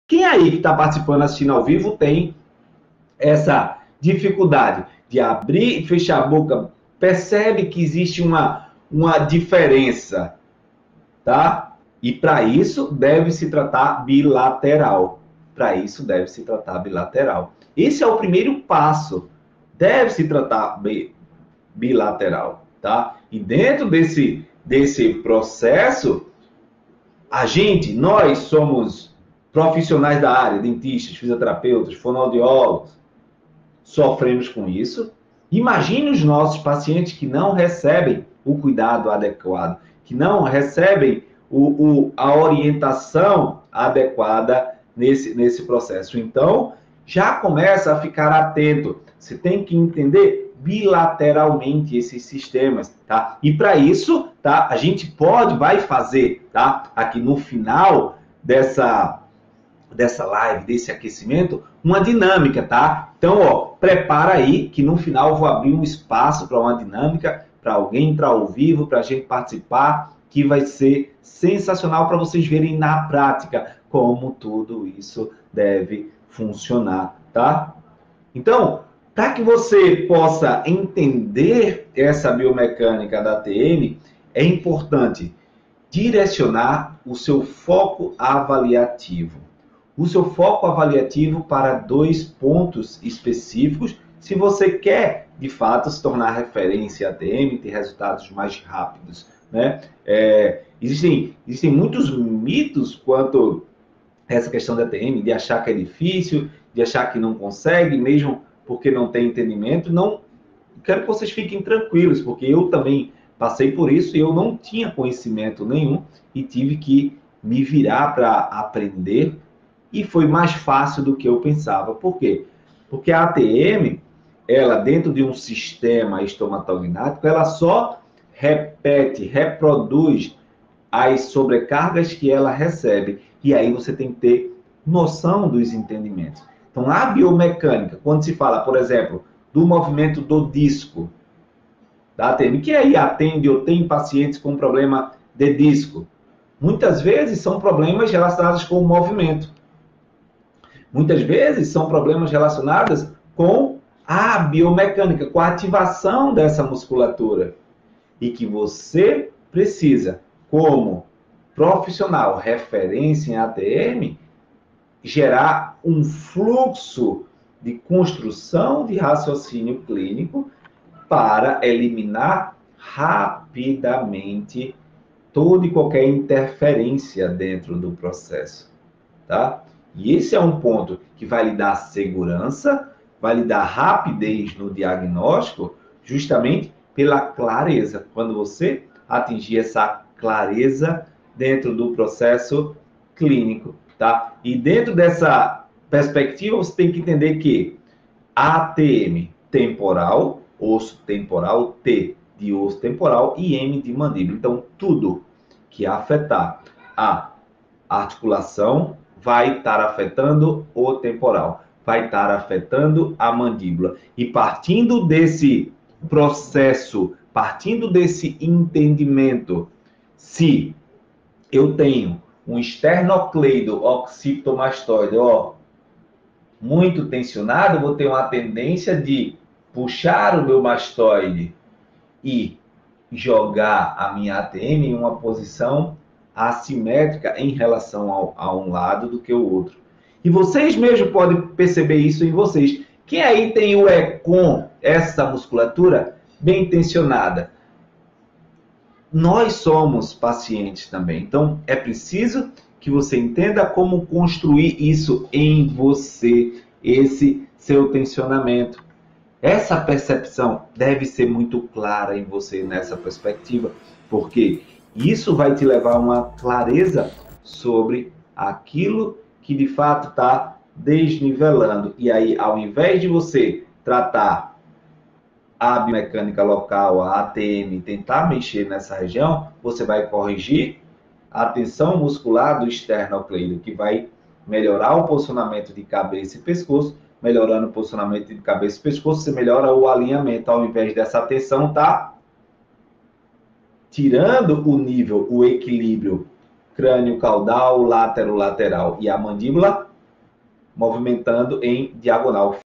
Pontos importantes para se tornar referência em ATM
Veja como fica mais fácil com esse raciocínio clínico conquistar melhores resultados na DTM

Integração é a palavra de ordem para tornar o tratamento mais eficaz, dessa forma o seu paciente vai lhe enxergar como referência e investir no plano de tratamento.

O crescimento profissional tem relação com o quanto o seu paciente percebe a sua competência, segurança e resultados.